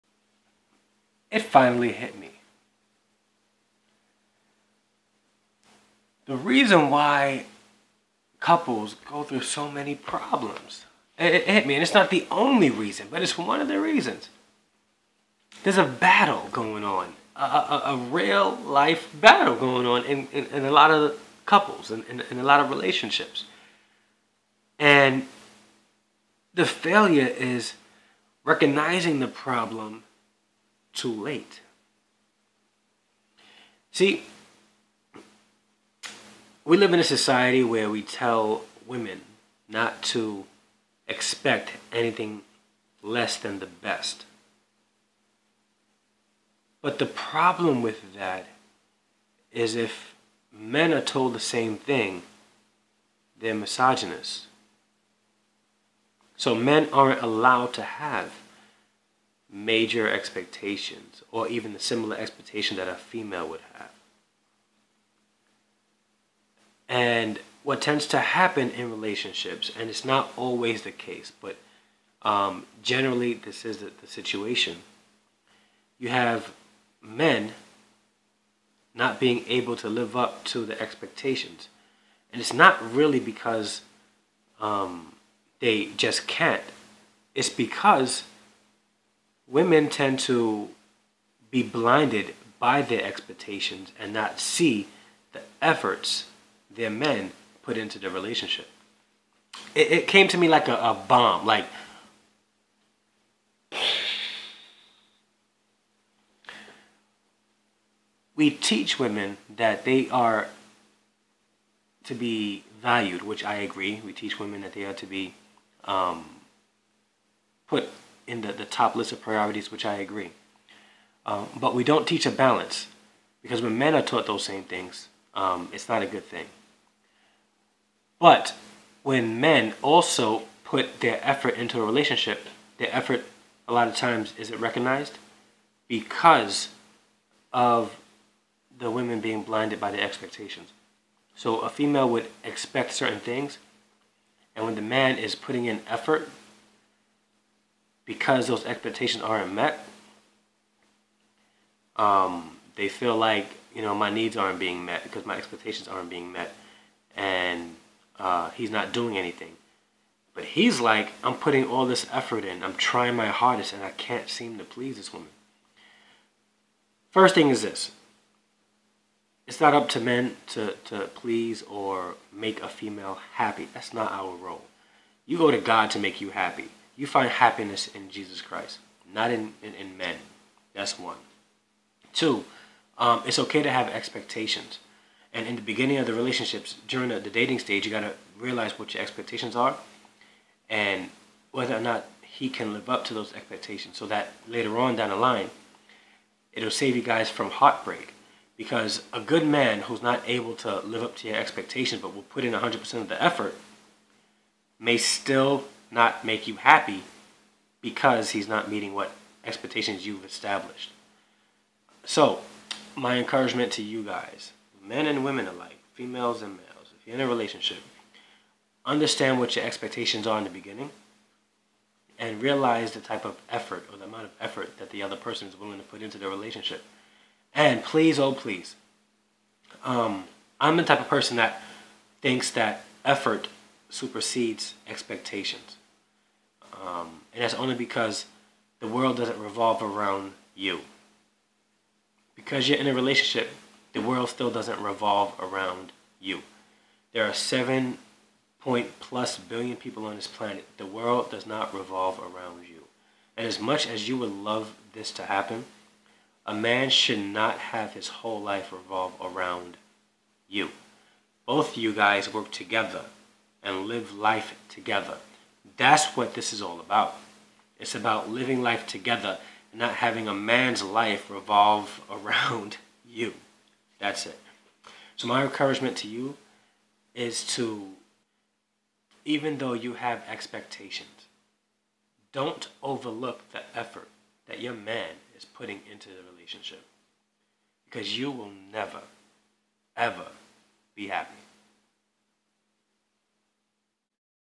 Efforts over Expectations
Do not let your expectations blind you from seeing the effort your spouse puts into the relationship.